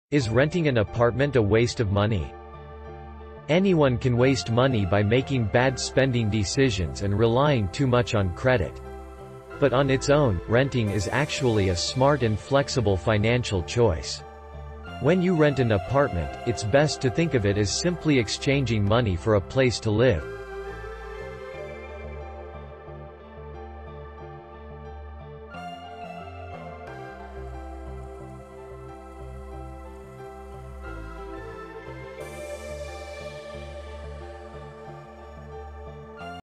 Is renting an apartment a waste of money?
Anyone can waste money by making bad spending decisions and relying too much on credit. But on its own, renting is actually a smart and flexible financial choice! When you rent an apartment, it’s best to think of it as simply exchanging money for a place to live.